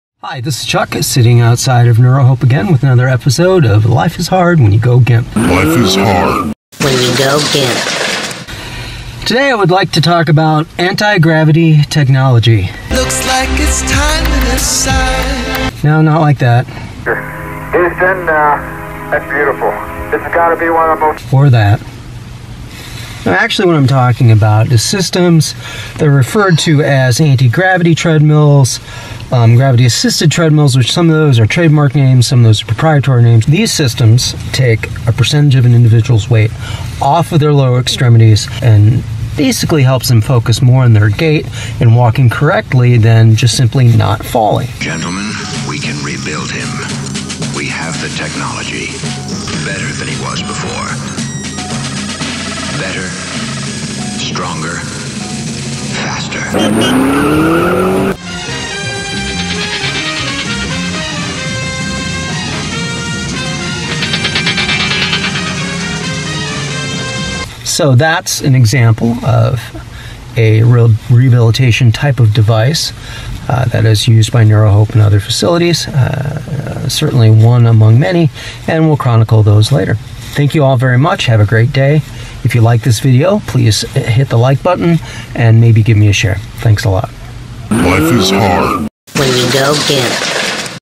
Treadmill training with body weight support.
Here's an example of a piece of equipment that helps individuals with SCI learn how to walk again.